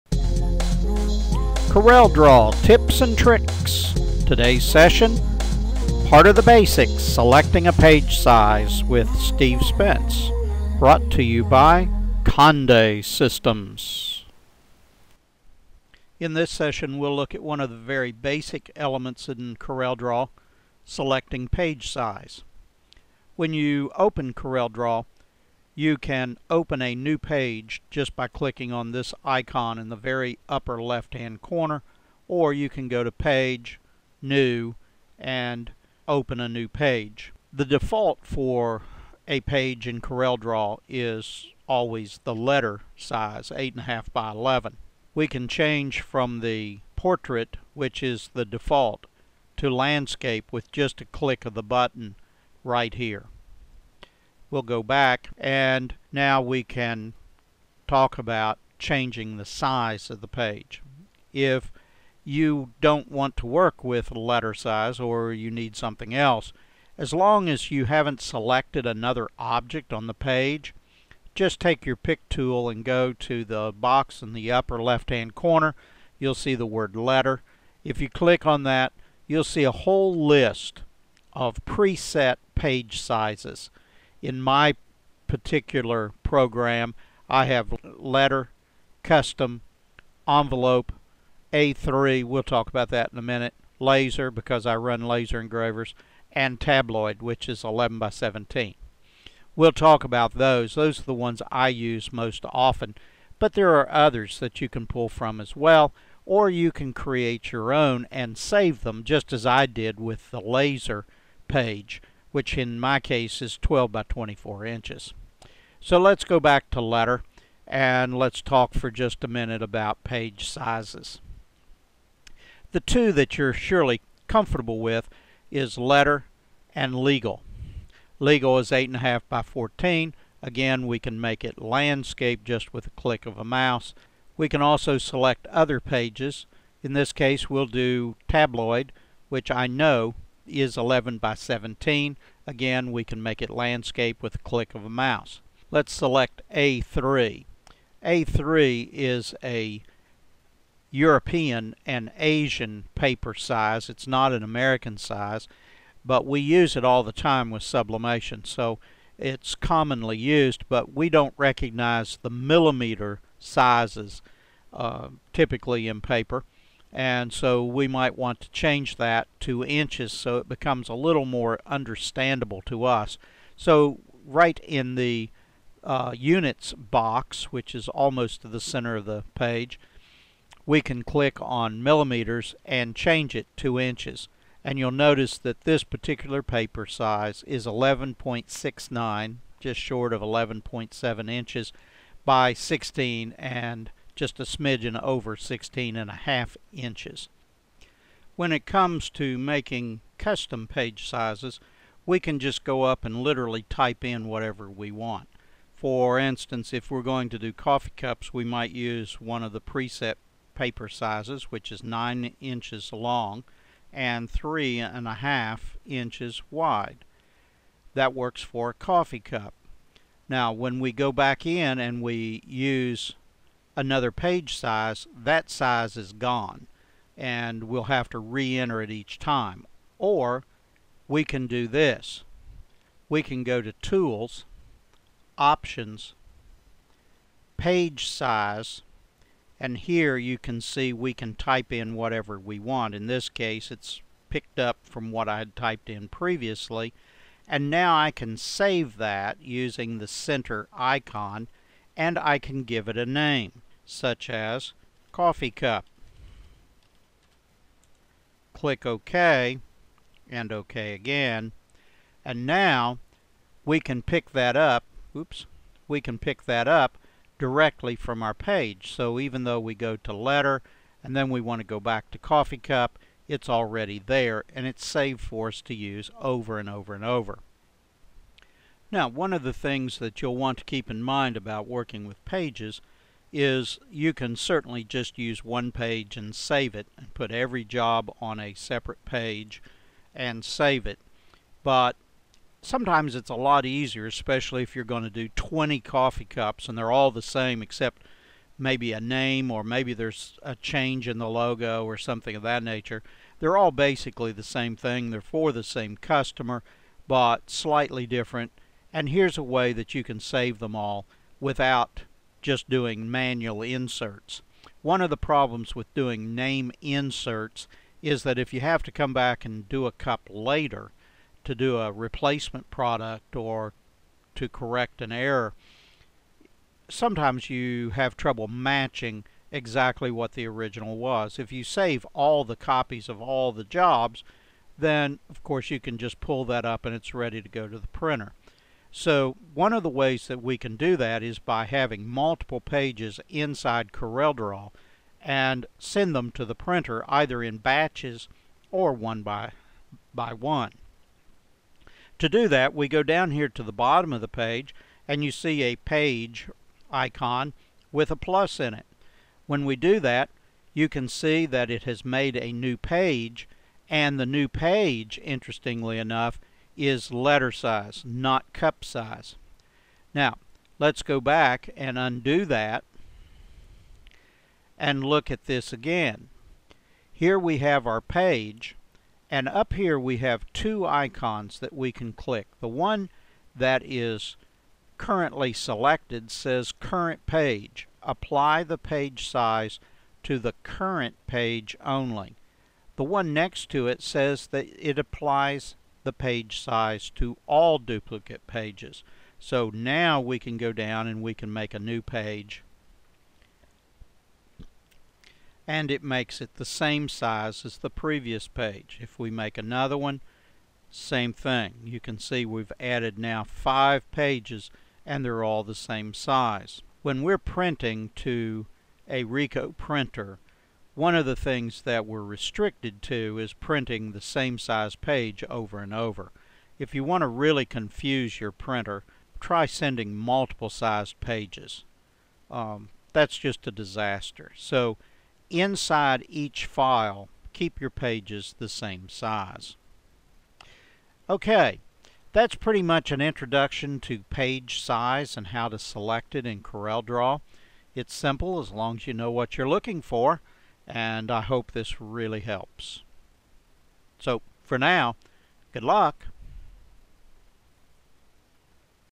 CorelDRAW Sublimation Tips: How to Select a Page Size -
CorelDRAW Tips & Tricks by Conde Systems Tech Support Blogger Steve Spence shows how to select a page size in CorelDRAW when preparing your image for digital imprinting with heat transfer or dye sublimation. This will work with almost any newer version of Corel. Graphic programs are essential to the imaged products industry and working with tools like Corel make the work easier and fun! Contact conde.com or call for information about working with personalized and imaged products created by dye sublimation and heat transfer applications.